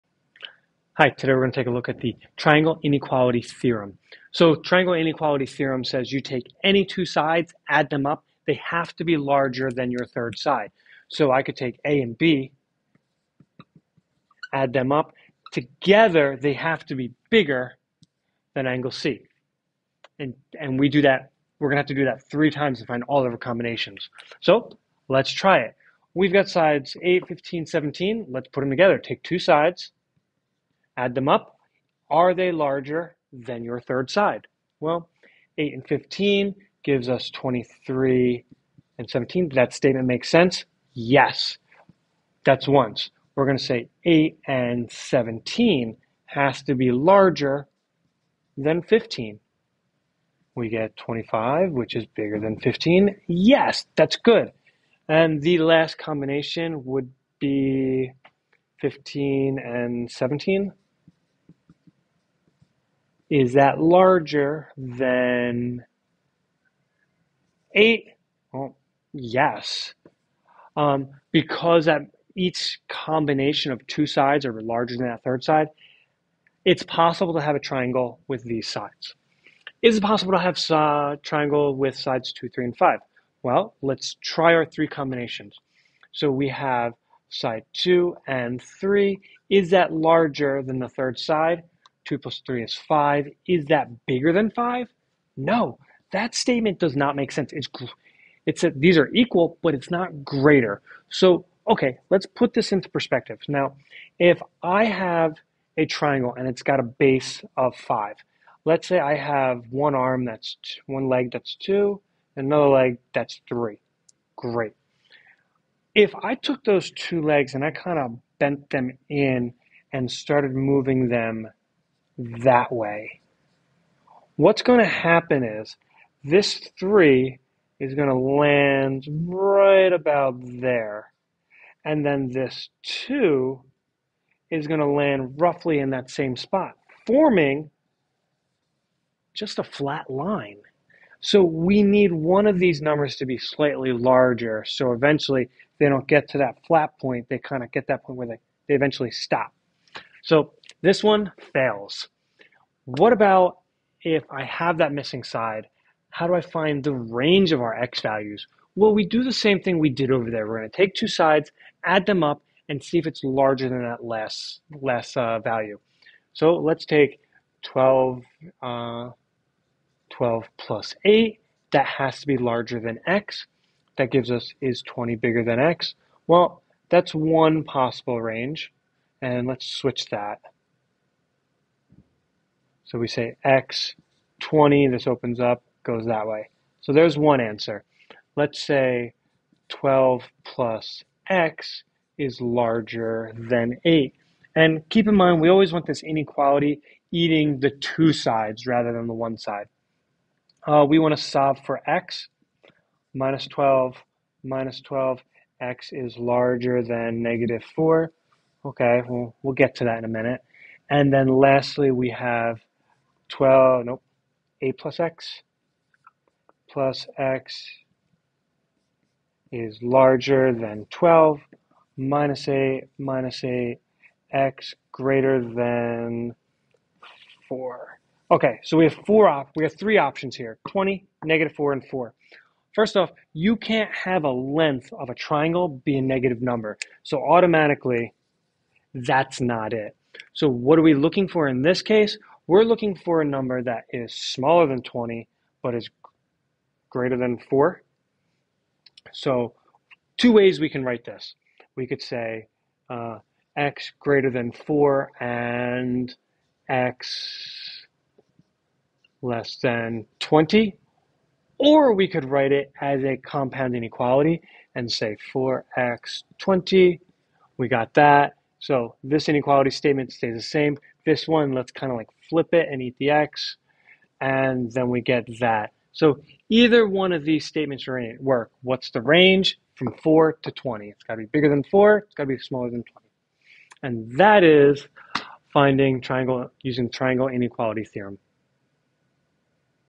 Geometry - Triangle Inequality Theorem
Geometry: Use the properties of the triangle inequality theorem to compare sides to verify if it is possible to create a triangle and find a possible range for a missing side.

Be kind to others, be empathetic, read a book, learn something new, respect nature, don't eat animals, and question everything.